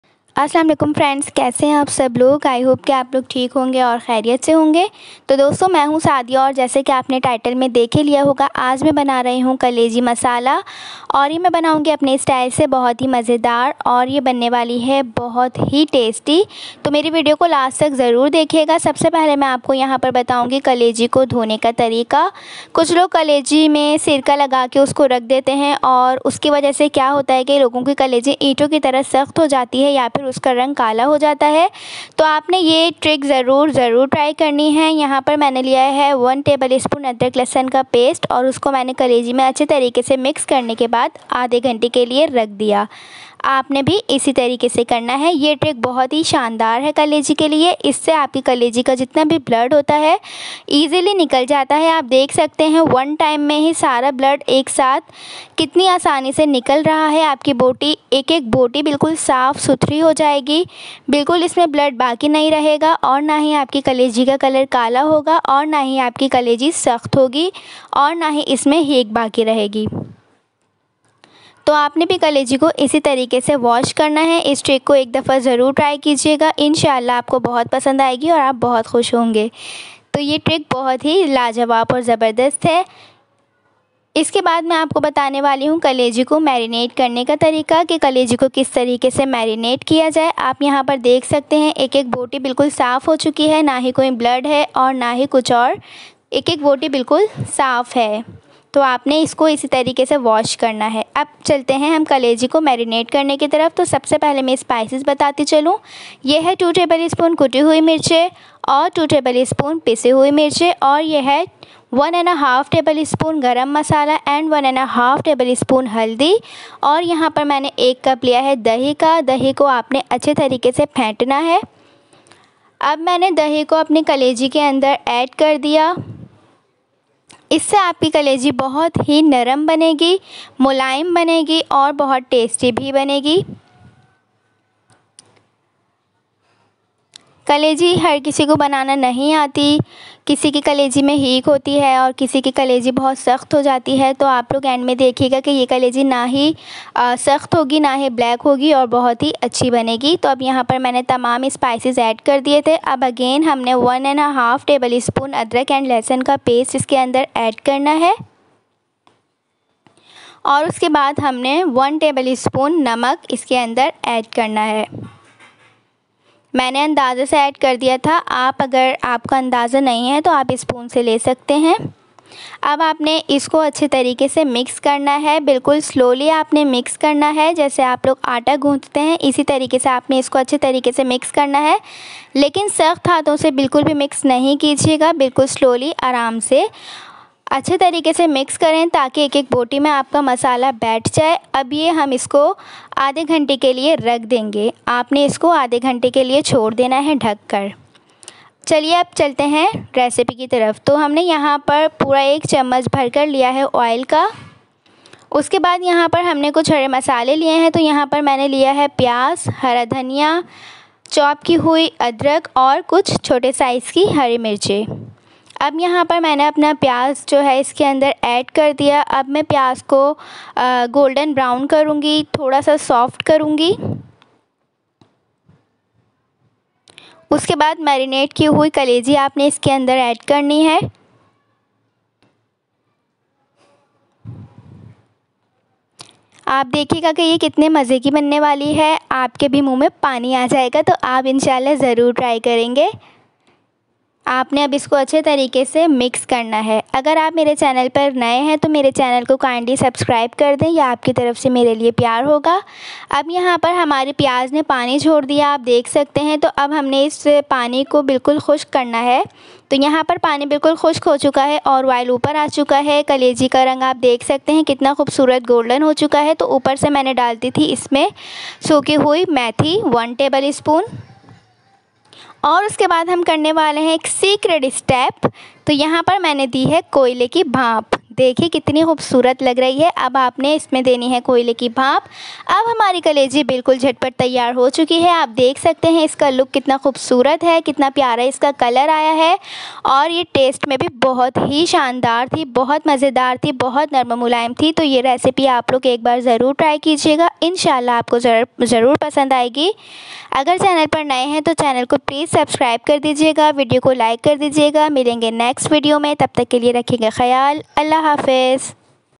Kaleji Masala Recipe|Unique Style
Kaleji Masala Recipe bht unique style me hai bht jaldi banny wali jhat phat ready to eat bht soft naram o mulaaen banti hai very tasty and delicious must try 😋